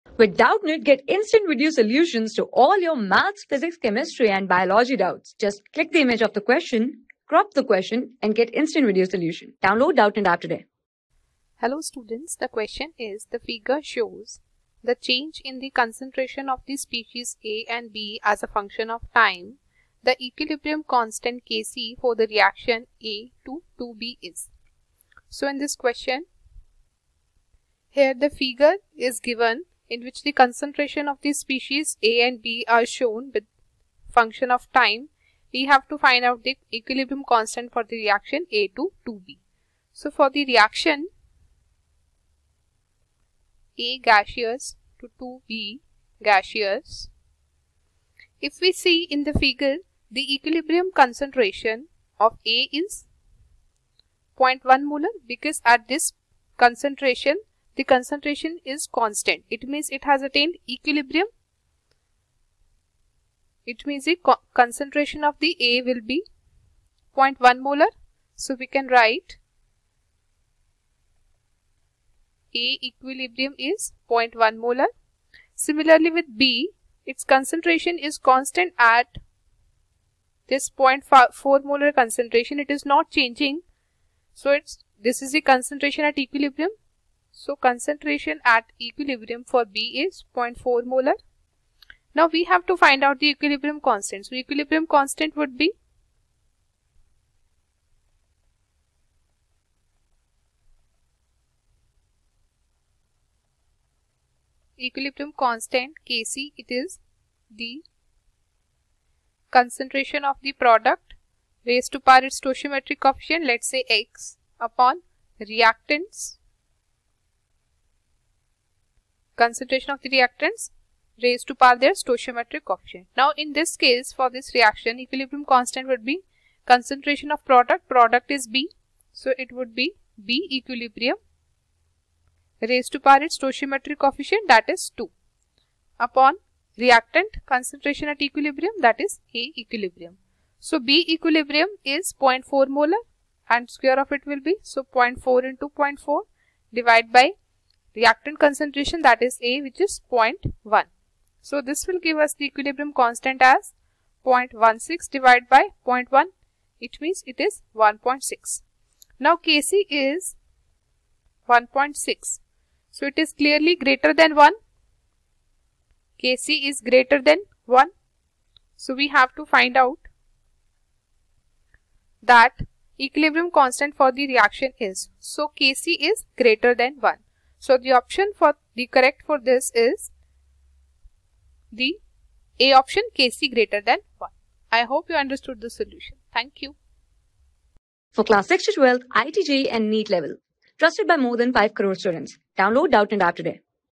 The figure shows the change in concentration of species A and B as a function of time. The equil...
The figure shows the change in concentration of species A and B as a function of time. The equilibrium constant K_(c)for the reaction A(g)hArr2B(g) is :
Class: 11
Subject: CHEMISTRY
Chapter: CHEMICAL EQUILIBRIUM
Board:IIT JEE

👉 Have Any Query? Ask Us.